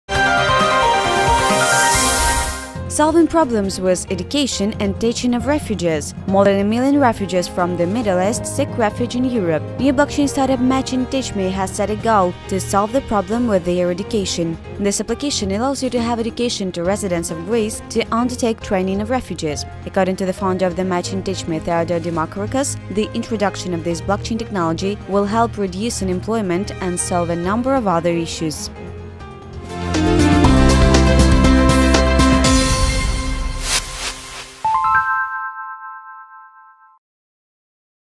KCN: Match & Teach Me
Solving problems with education and teaching of refugees. More than a million refugees from the Middle East seek refuge in Europe. New blockchain startup Match & Teach Me has set a goal to solve the problem with their education. This application allows you to have education to residents of Greece to undertake training of refugees. According to the founder of the Match & Teach Me Theodor Dimakarakos "The introduction of this blockchain technology will help reduce unemployment and solve a number of other issues."
EDR: anna-profit
BTC: 1JdRXQuqicwSyn61cd9jyXzAk2QUfu4KEW